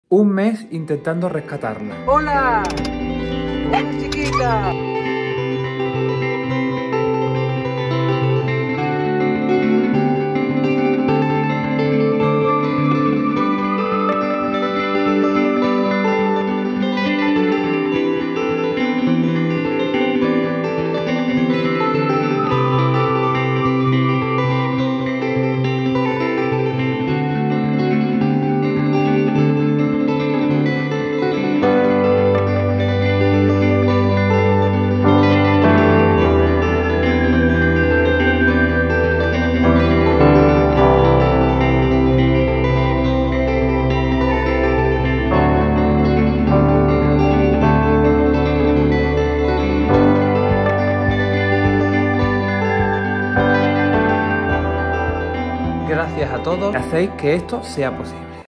🔵Final feliz para la PODENCA Lulú Podencos rescatados
Tras mucho sufrímiento y esfuerzo, Lulú descansa a salvo ❤️

Puedes apoyar mi trabajo, púlsando en la pestaña donde pone GRACIAS, deslizando hacia la derecha donde están los pulgares del Like. 👍 👎 ➡️

Transferencia: ES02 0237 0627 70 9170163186. Gracias ❤️